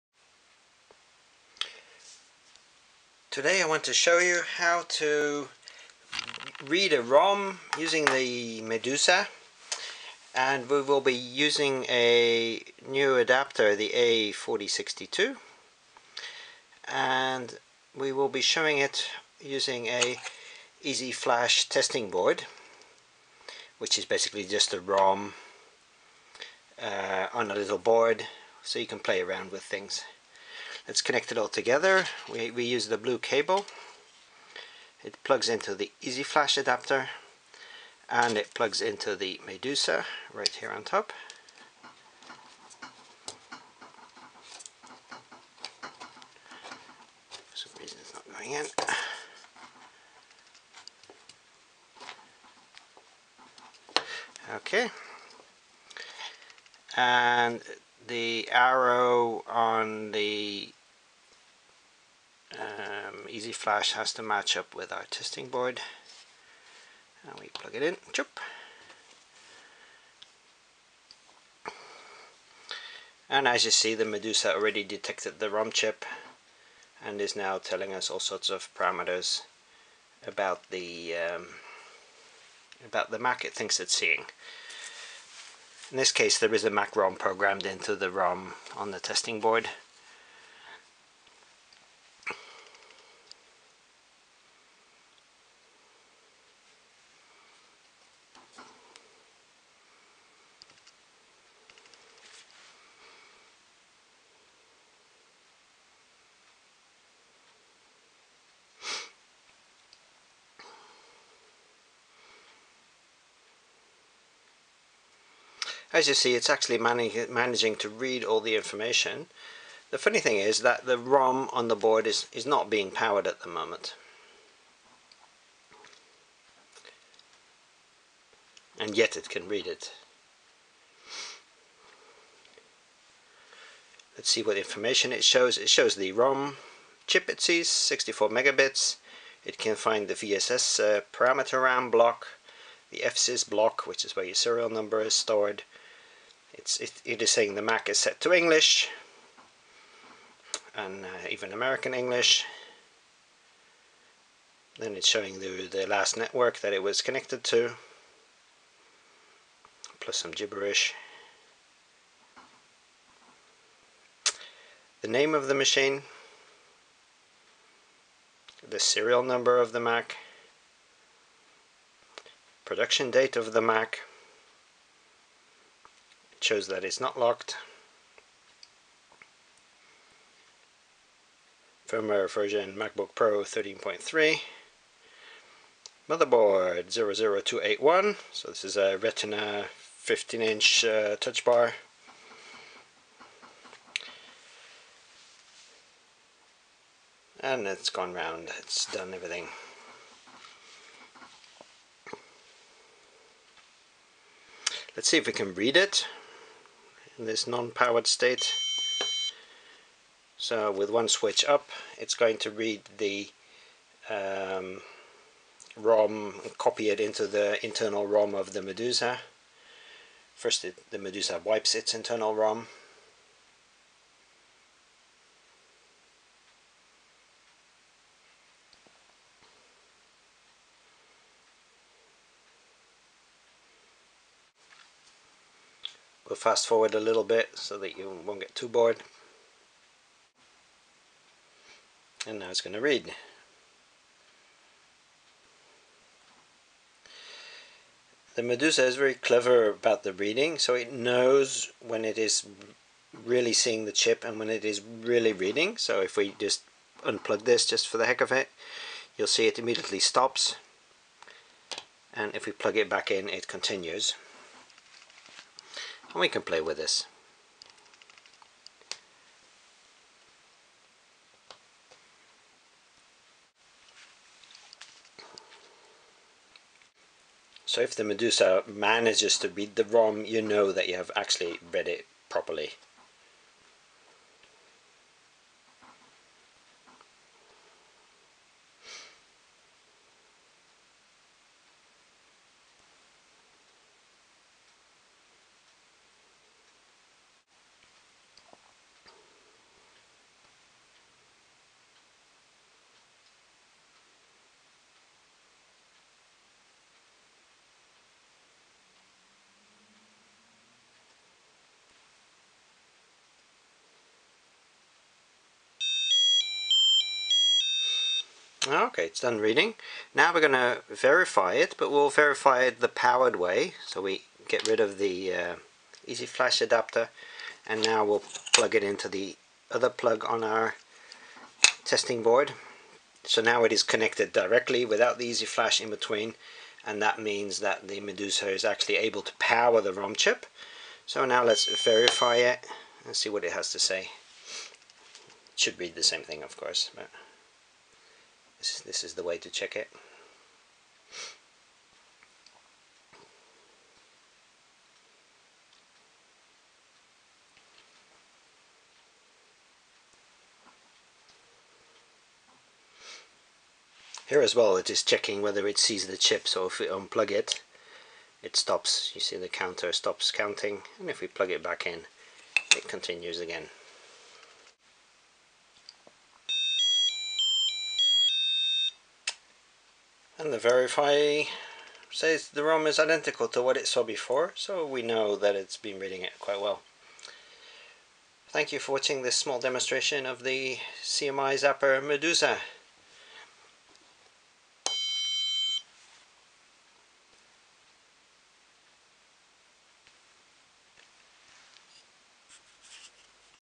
CMIzapper Medusa 2 to read the ROM from the testing board
Using the CMIzapper Medusa to read an EFI ROM from the Easy Flash testing board using an Easy Flash adapter.